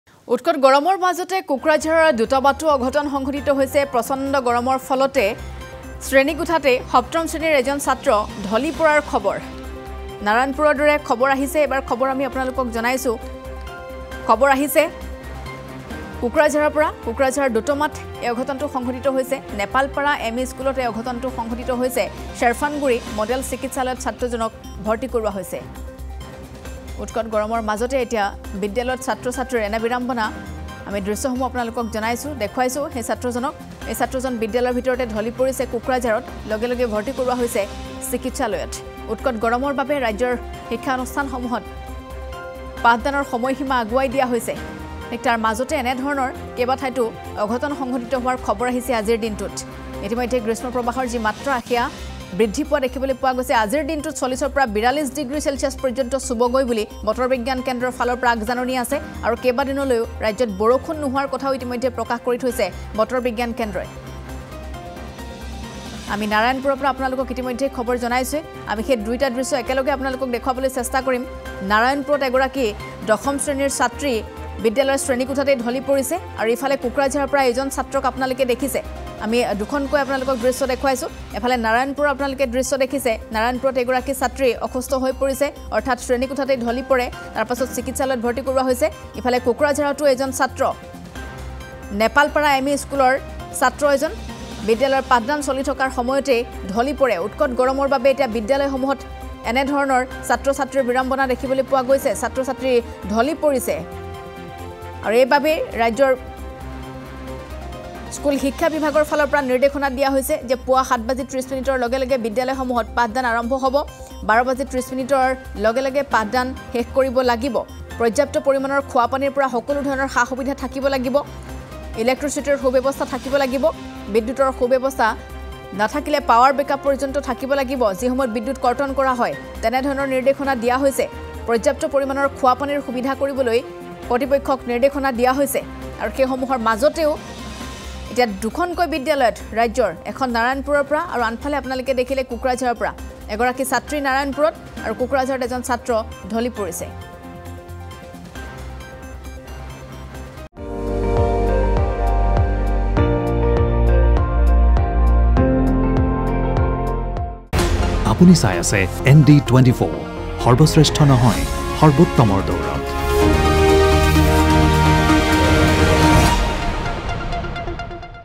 Assam Hot Weather: উত্কট গৰমৰ মাজতে কোকৰাঝাৰৰ দোতমাতো অঘটন
উত্কট গৰমৰ মাজতে কোকৰাঝাৰৰ দোতমাতো অঘটন। প্ৰচণ্ড গৰমৰ ফলত শ্ৰেণীকোঠাত ঢলি পৰিল সপ্তম শ্ৰেণীৰ ছাত্ৰ। নেপালপাৰা এম ই স্কুলত এই অঘটন। ছেৰফাংগুৰি মডেল চিকিত্সালয়ত ভৰ্তি ছাত্ৰজনক।